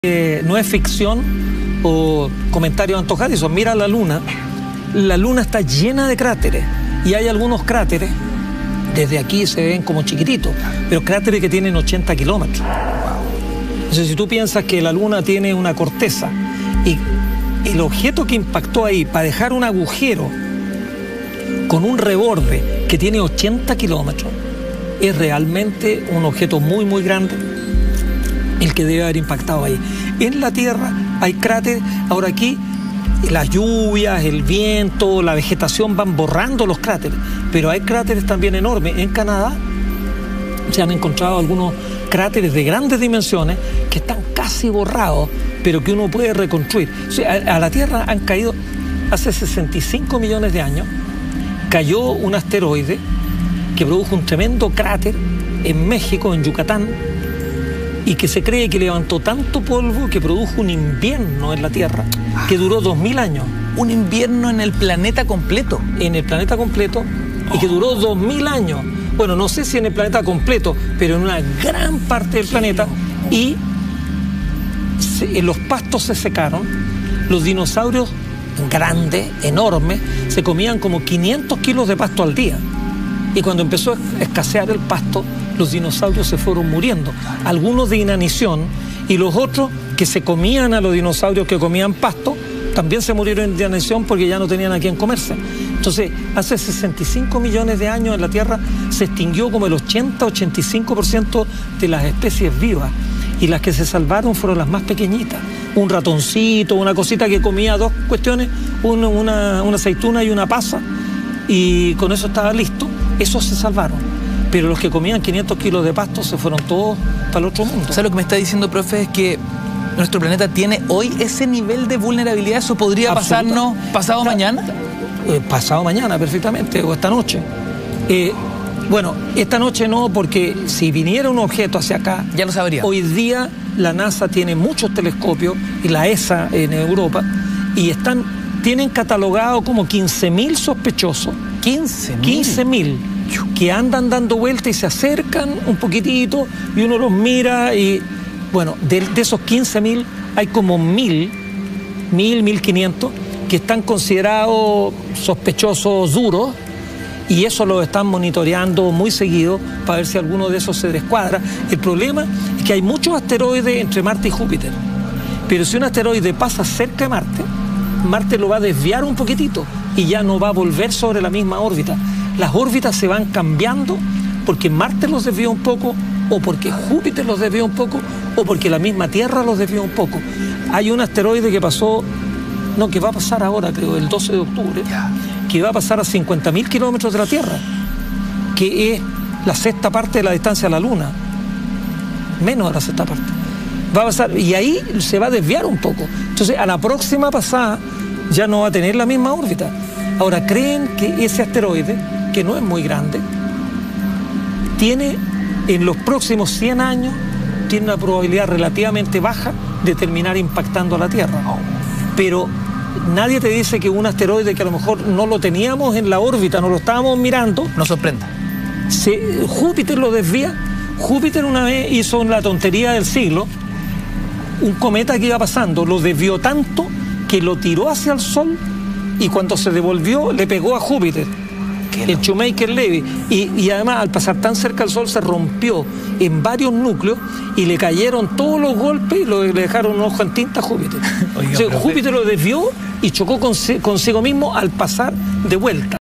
¿Podría un asteroide destruir nuestro planeta?
El profesor José Meza habló de los peligros que tendría el planeta Tierra ante la posibilidad de ser impactado por un asteroide.